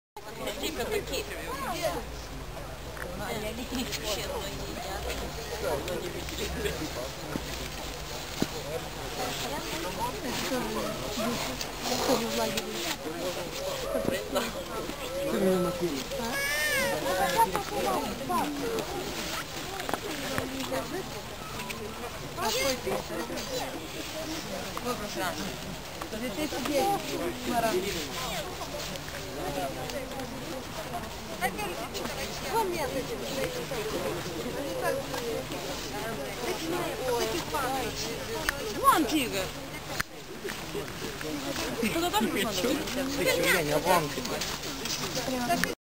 Bălţi - Statui de gheaţă
O expoziţie de statui sculptate în gheaţă a vernisat azi, 18 ianuarie, la ora 16, companiile StarNet şi NordLinks la Bălţi. Expoziţia va dura până pe 21 ianuarie curent.
Recent, StarNet a vernisat o expoziţie similară şi în centrul Chişinăului.